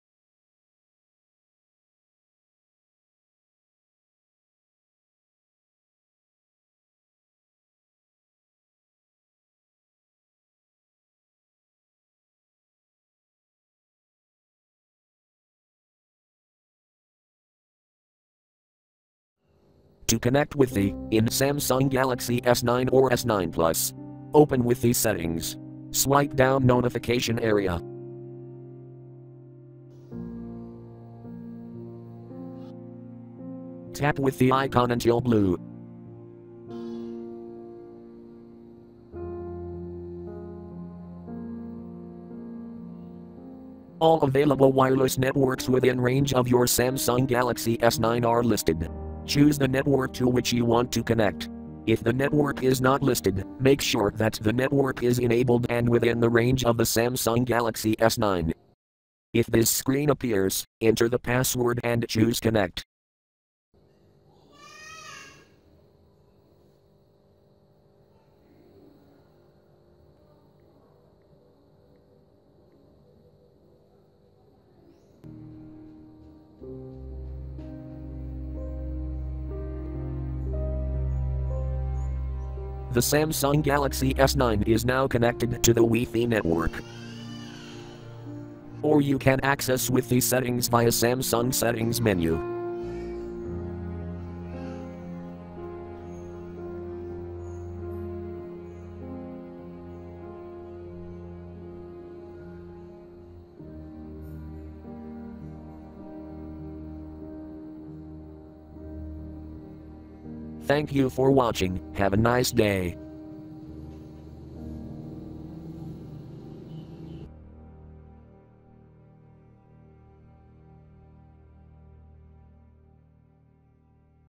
Samsung Galaxy S9 : How to connect Wifi (Android Oreo)
This video show How to connect Wifi in Samsung Galaxy S9 or S9+.  In this tutorial I use Samsung Galaxy S9 SM-G960FD International version with Android 8.0 (Oreo).